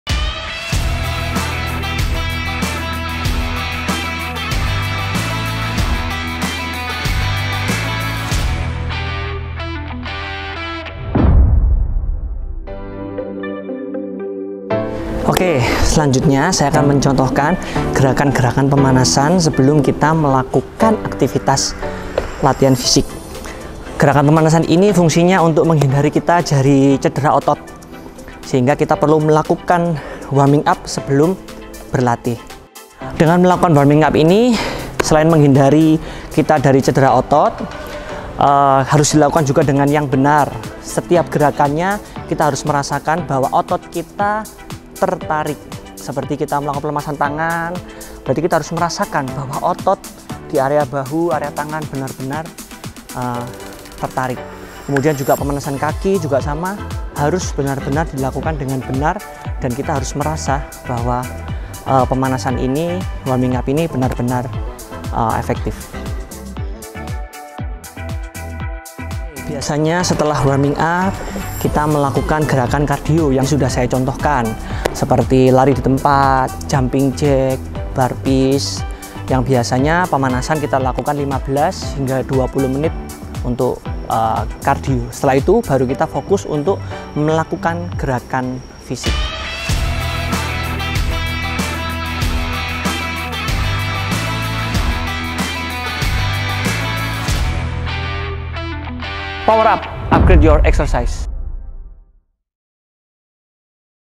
PowerUp ep 7 - Pentingnya Pemanasan
Kendaraan aja perlu dipanasin sebelum digunakan, masa tubuh kita enggak?

Kalian tahu kan, kalau pemanasan sebelum berolahraga itu sangat penting bagi tubuh kita sebagai persiapan untuk melakukan gerakan yang berat. Pemanasan berguna untuk mencegah cedera yang bisa dialami saat berolahraga. Tapi apakah kita sudah melakukan pemanasan dengan benar?

Nah, kali ini dalam PowerUp, Ricch akan membagikan cara melakukan pemanasan yang benar, sehingga tubuh kita siap untuk berolahraga.

PoweUp! Upgrade your excercise!